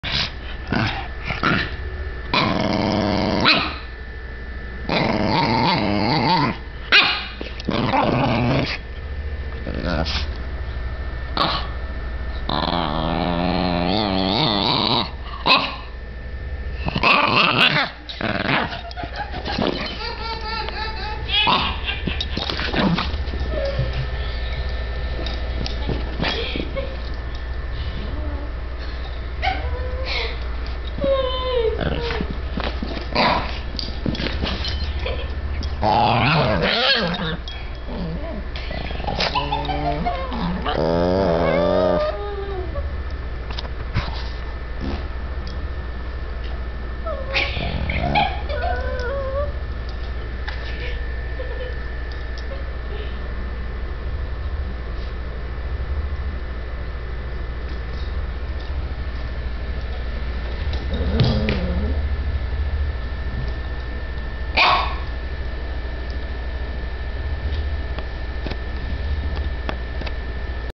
Zoe and Mojo playing 2
Our Boston terrier (Zoe) and Chihuahua (Mojo) like to fight over the same toy. Zoe gets worked up, because Mojo stands his ground. BTW Zoe's bark is hilarious!!